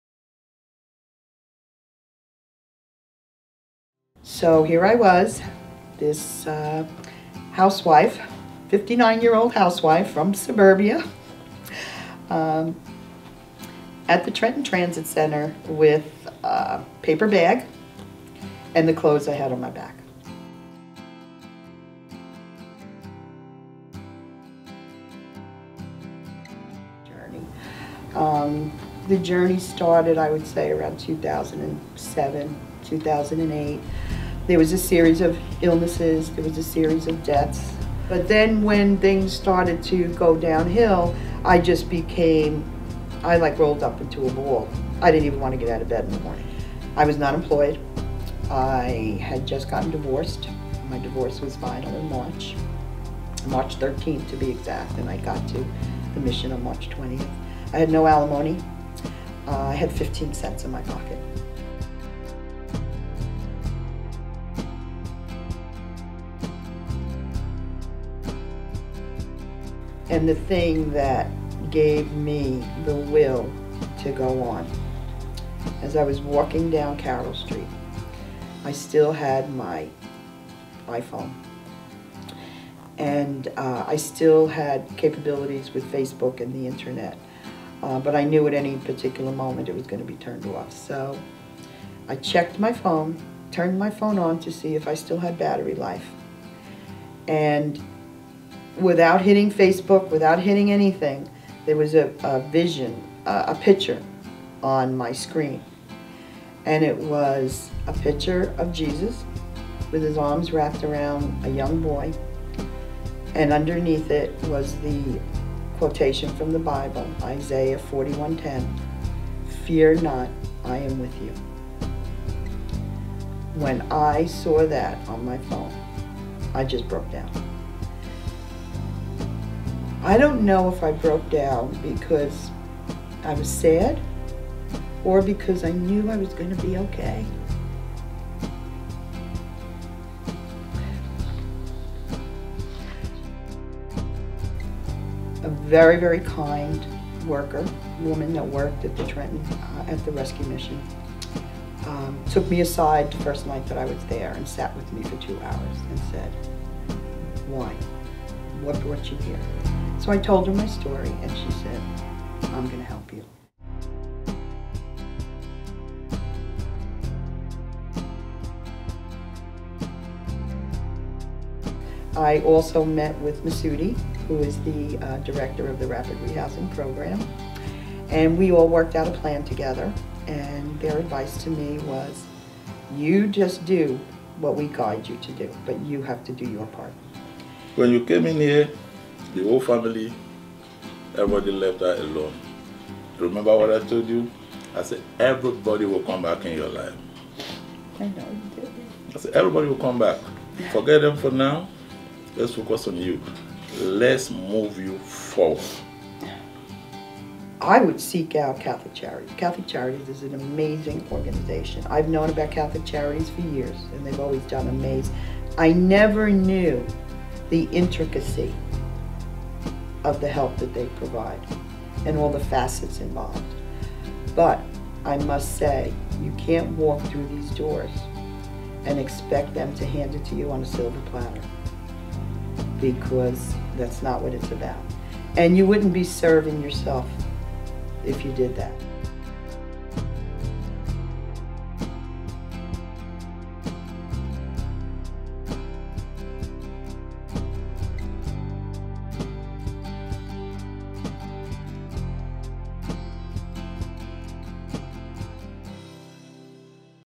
Donna's Story of Hope
This is Donna, her journey actually began in 2007 with a gradual and steady series of losses, starting with emergency surgery, 18 months on strong pain medications, the deaths of her mother and closest friend. Listen to her story of recovery and hope as she discusses her journey from homelessness to resuming a new life.

Donna is walking in our Walk of Hope this April 5th and you can too!